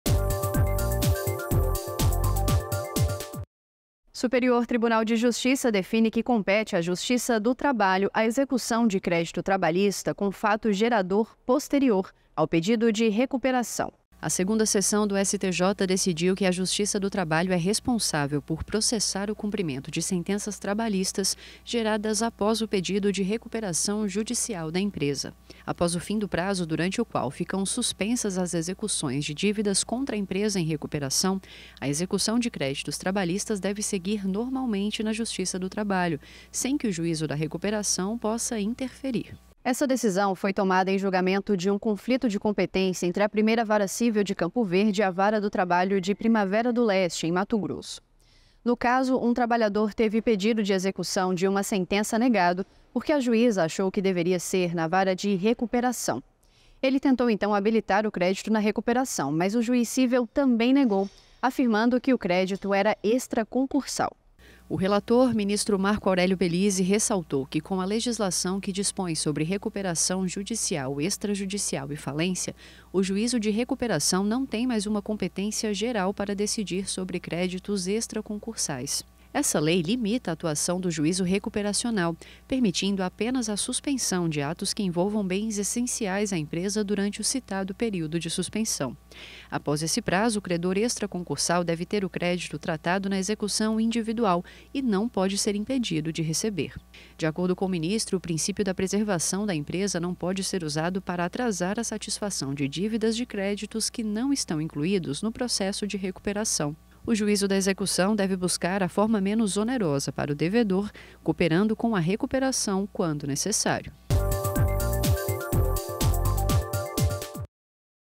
Compete à Justiça do Trabalho a execução de crédito trabalhista gerado após pedido de recuperação
​A Segunda Seção do Superior Tribunal de Justiça (STJ) decidiu que é da Justiça do Trabalho a competência para processar o cumprimento de sentença trabalhista cujo crédito teve seu fato gerador em data posterior ao pedido de recuperação judicial.
Para o colegiado, após o fim do stay period (prazo durante o qual ficam suspensas as execuções de dívidas contra a empresa em recuperação), a execução do crédito trabalhista extraconcursal deve prosseguir normalmente perante o juízo trabalhista, sendo vedado ao juízo da recuperação controlar os atos constritivos daquele processo, pois a sua competência se limita ao sobrestamento de ato constritivo que incida sobre bem de capital.